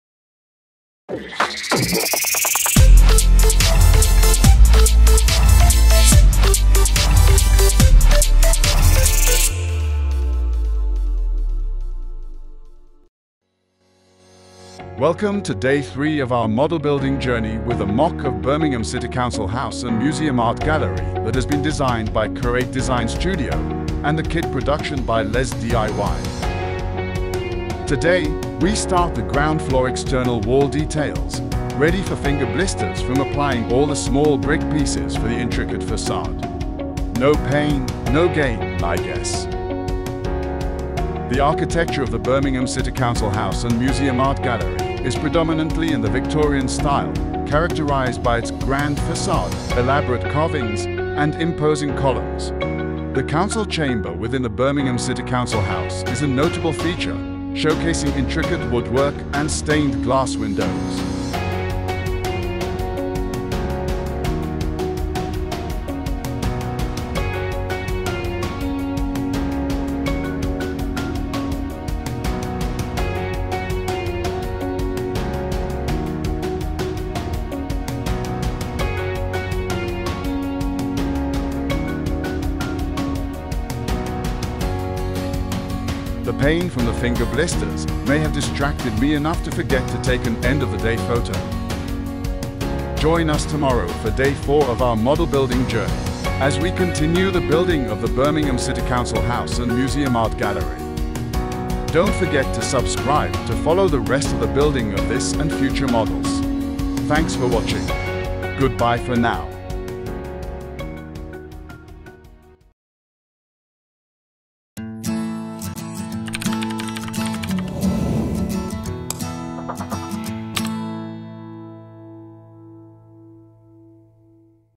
Mega Model Build Day 3
Day 3 of the building of the Birmingham City Council House & Museum Art Gallery brick model.